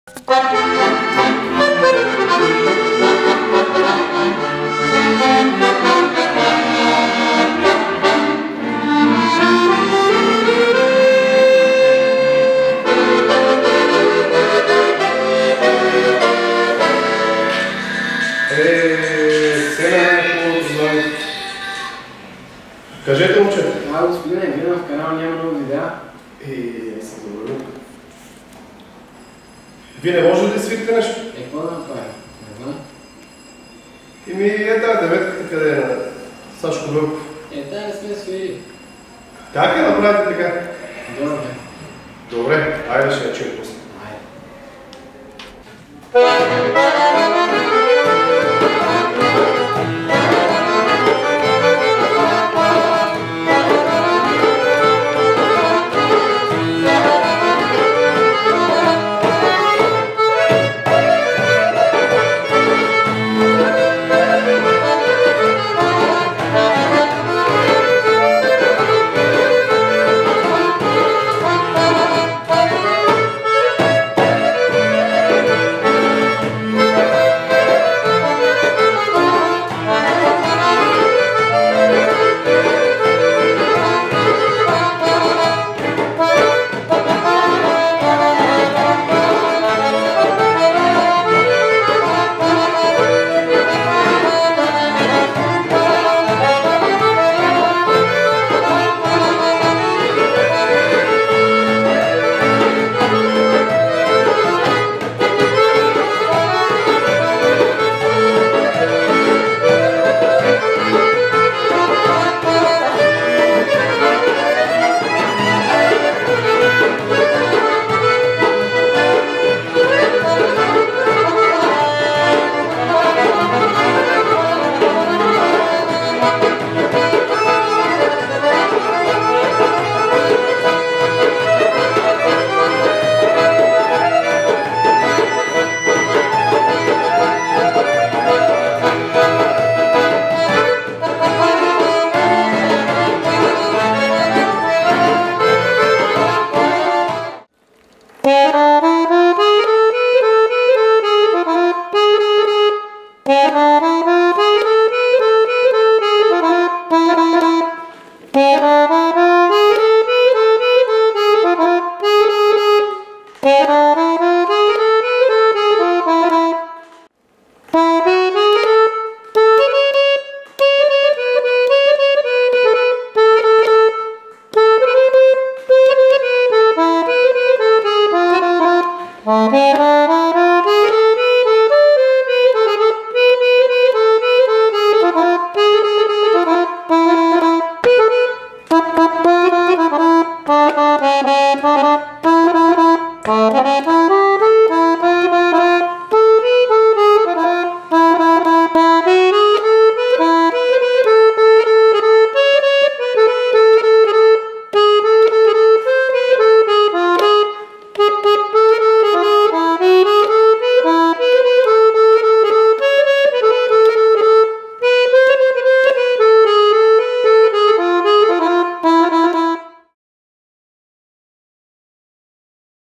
Tutorial 9 - 9/8 (Sashko Velkov)
Stoqn Dulgerov, Miroslav Karatanchev and Kristiqn Vakadinov -  9/8 Makedonsko